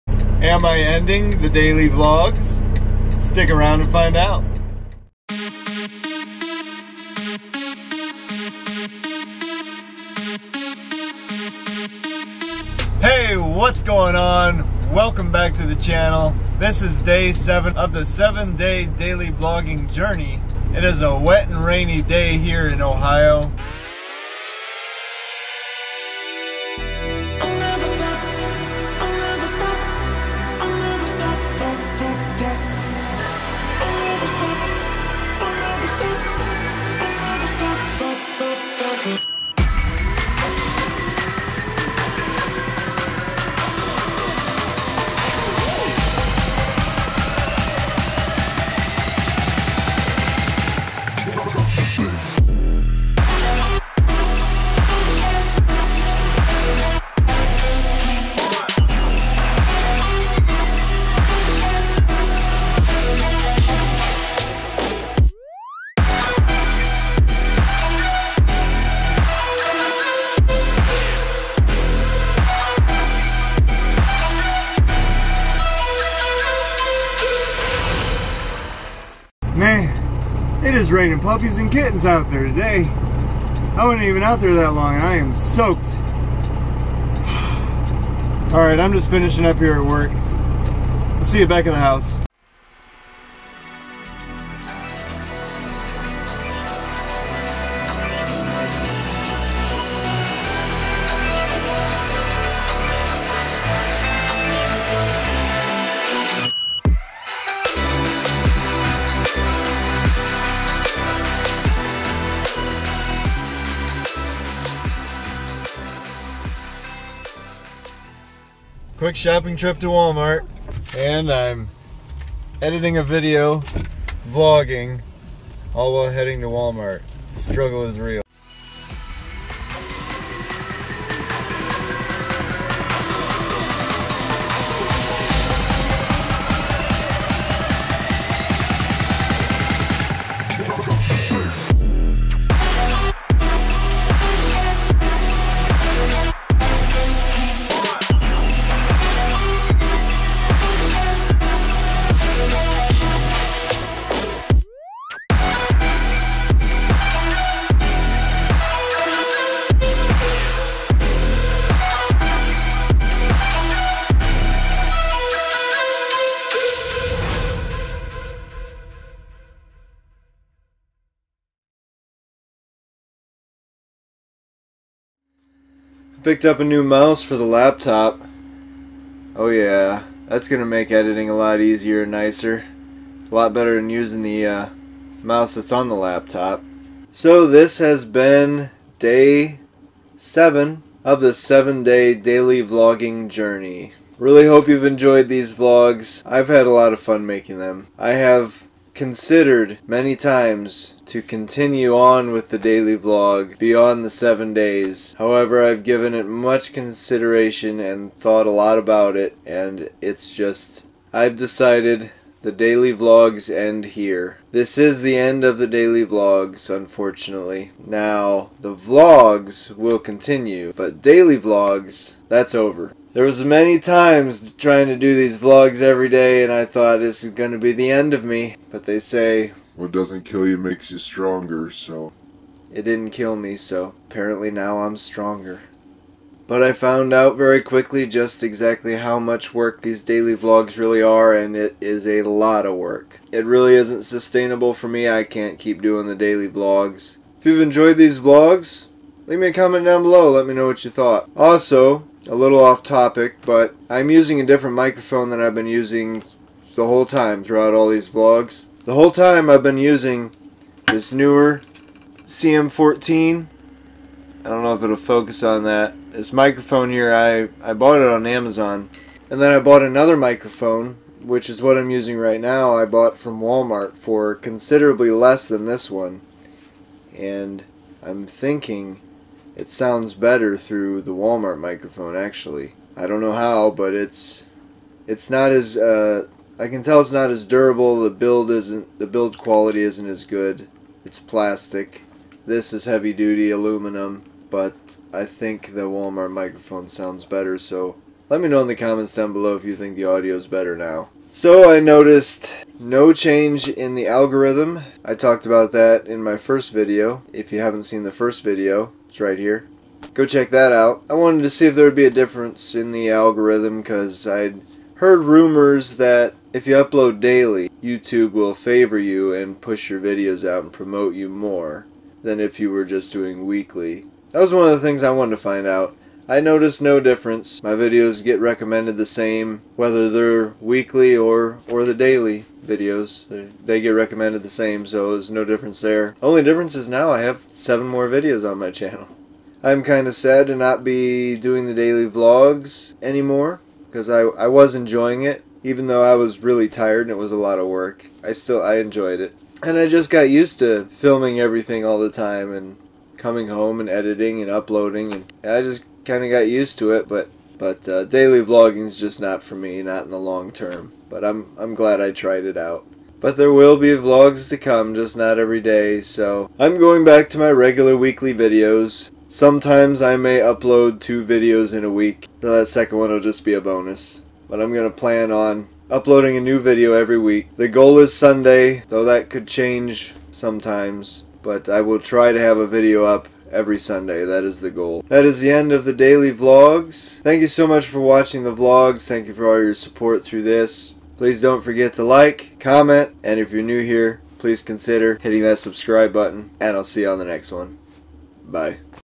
Ending the daily vlogs?
Day 7 of my 7 day, daily vlogging journey. Is this the end of the daily vlogs? Watch this video and find out! Daily vlogging has been so much fun and a lot of work. I have learned so much throughout this daily vlogging journey.